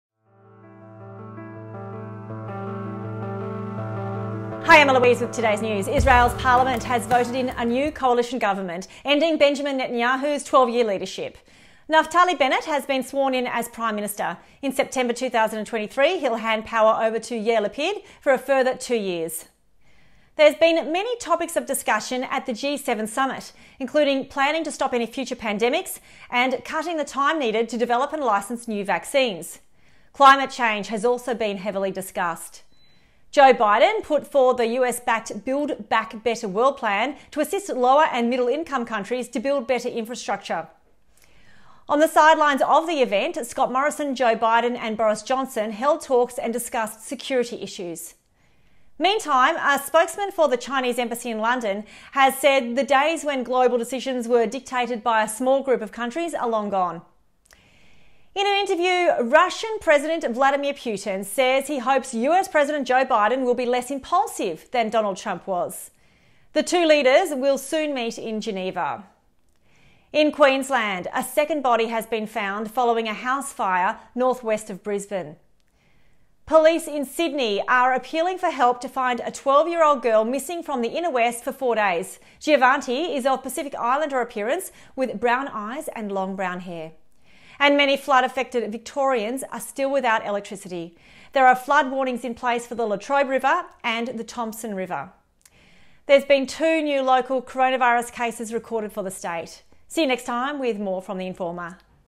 TheInformer News Update | 14 June 2021
Eloise Grace with the biggest headlines from around the world including big news out of Israel and Victoria's latest COVID numbers.